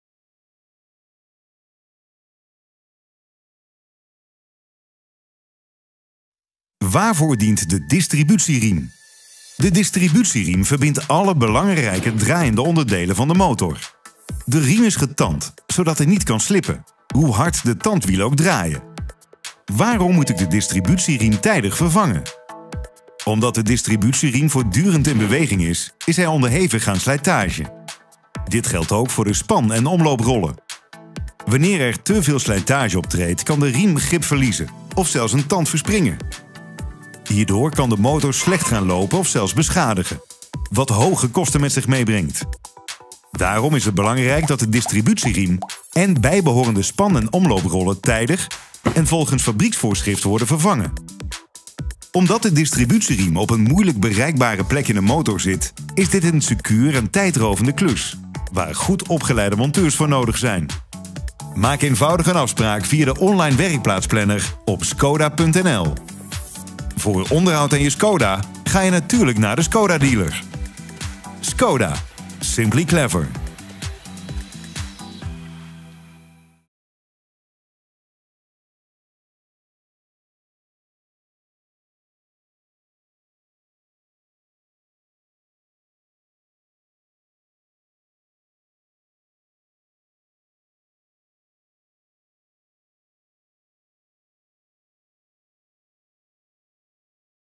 ŠKODA Distributieriem
De distributieriem van jouw ŠKODA is enorm belangrijk voor de motor. In deze video vind je alle informatie met betrekking tot de distributieriem. Denk jij dat uw distributieriem toe is aan vervanging? Maak een online afspraak of kom eens langs bij een van onze vestigingen in 's-Hertogenbosch of Veldhoven.